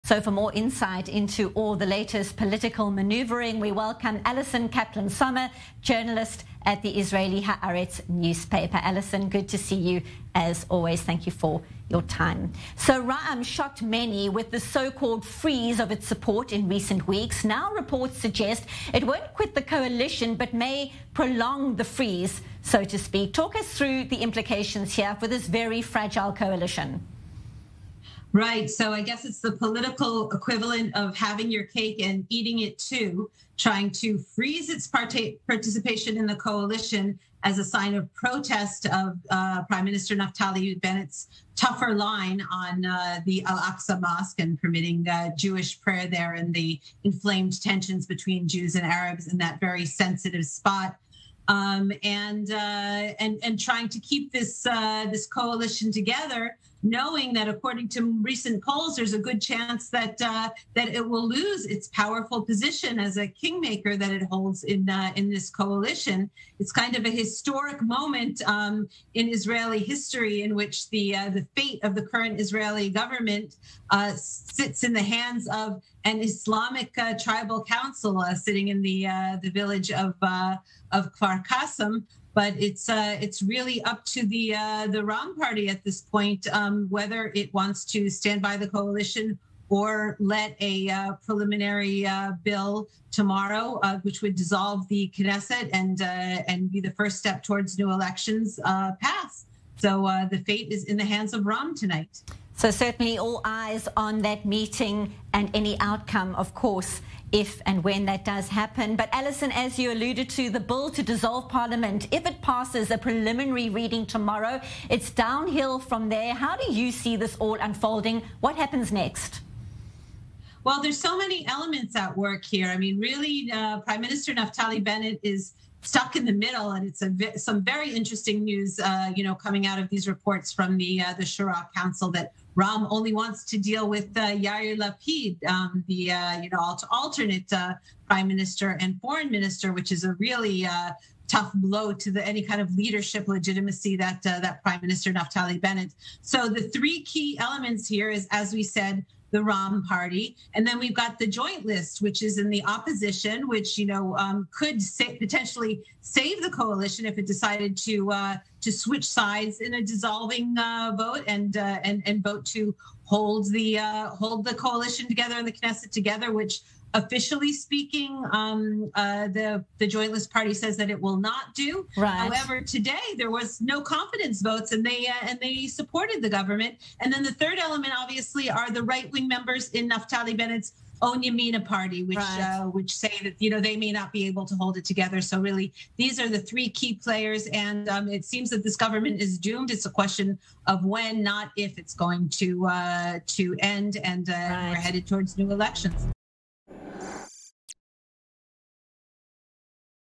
Ra'am party meets about future in coalition
Ra'am shocked many by its freeze of support for the current government, however, reports suggest it will not quit the coalition but may prolong the freeze.

"It is a historic moment in Israeli history that the fate of the current Israeli government sits in the hands of an Islamic tribal councilor, " Allison Kaplan Sommer, journalist at Haaretz, told i24NEWS